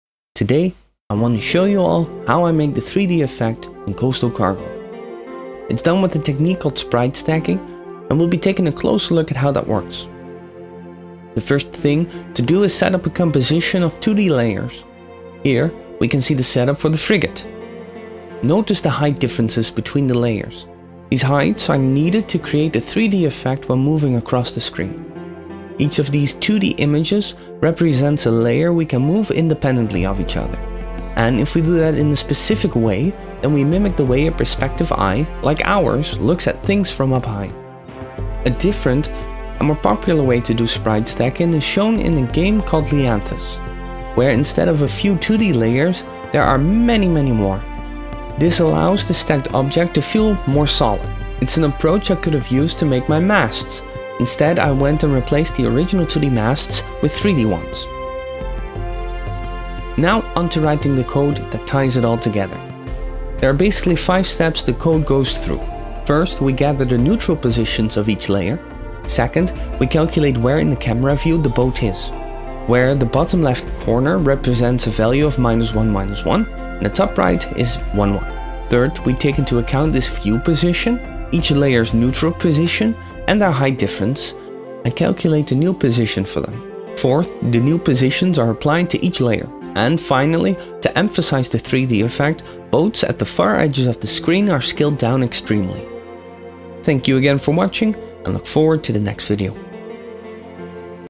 What is Sprite Stacking and how does it improve your life?!?
How to create fake 3D using 2D images, now finally explained.
Add yourself: Specific implementation